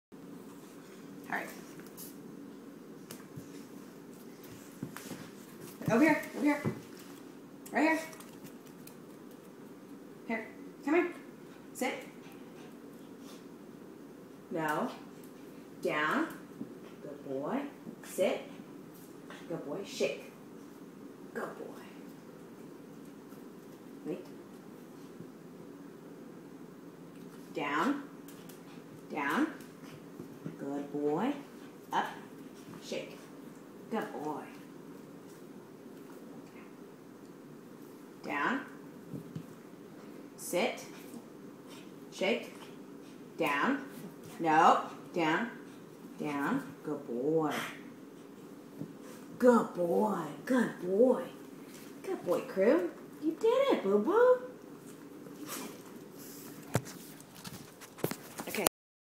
Shih tzu being a good boy
My shih tzu being a good boy. His name is Crew :-)